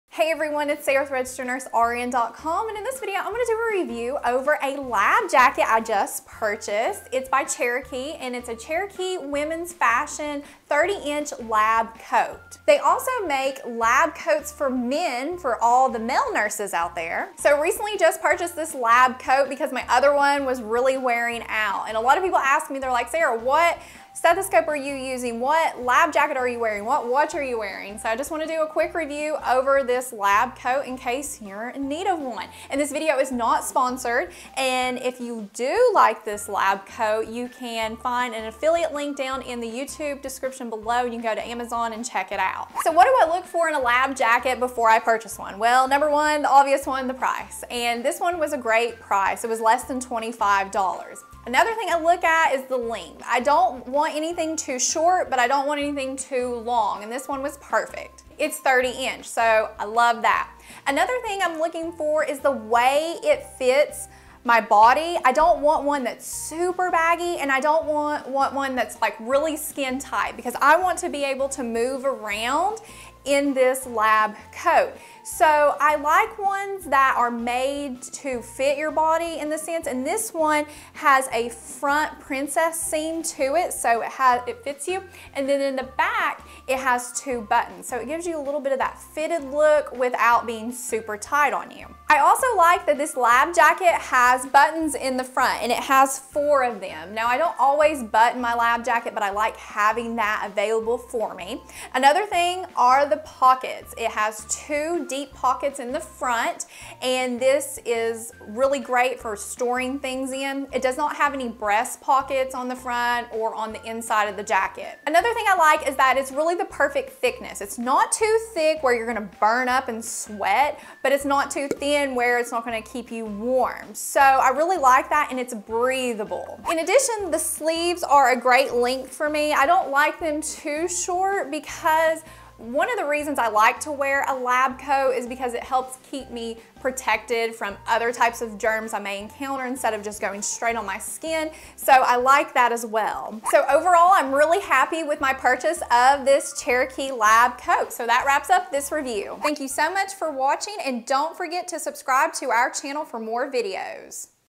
My New Nursing Lab Coat: Cherokee Women's Fashion Lab Coat Review
New nursing lab coat review with tips. So, after shopping around online for different lab jackets, I eventually purchased the Cherokee Women’s Fashion Lab Coat. I think this nursing lab coat is super cute.

Overall, I’m very happy with my purchase. I think this jacket is super cute, and for you male nurses out there, they also carry men’s lab coats as well.

Here are the features I prefer in a lab jacket, which is why I decided to buy this particular style and brand:
• It was very affordable. I paid less than $25 for this lab jacket, which was a great deal considering the quality and fit.
• It is 30” in length, which is just right for me. I don’t like lab jackets that are too short or too long. At 30”, I felt that it was just right.
• I love the way this lab jacket fits overall. I don’t like to wear anything too tight, because it feels too restrictive when I’m moving around. Likewise, I don’t like anything too baggy, either. This lab coat fit me just right.
• The front princess seams give this jacket a fitted look. In addition to the overall fit, the princess seams in the front give the jacket a fitted or tailored look, which I think makes it look more stylish/cute.
• The waist is adjustable. There are two buttons on the back of the lab jacket’s belt that allow you to adjust the waist, for an even more comfortable fit.
• It has a total of four buttons in the front. This is a great feature, because some lab jackets don’t have buttons or zippers. Although I don’t always button my lab jacket, I do like having the option available.
• It has two deep pockets in the front. I’m a sucker for pockets in scrub tops and lab jackets. I have always placed my nursing supplies in my pockets. This lab jacket has very nice, deep pockets that will allow you to store lots of supplies in them.
• The material is just the right thickness. This lab jacket is thick enough to keep you warm, but at the same time, it’s not so thick that you’ll burn up in it. The fabric is also breathable, and it is made of 65% polyester and 35% cotton.
• The sleeves are just right. I like long sleeves on a lab coat, primarily to prevent germs or bodily fluids from contacting my skin.
• On the downside, this particular lab jacket does NOT have a breast pocket or inside pockets, which some nurses might prefer, but that wasn’t a deal breaker for me.
Overall, I have always preferred to wear lab jackets as a nurse, and I was very happy with the Cherokee Women’s Fashion lab coat. It fits great, looks super cute, and has nice, deep pockets in the front, all at a great price point.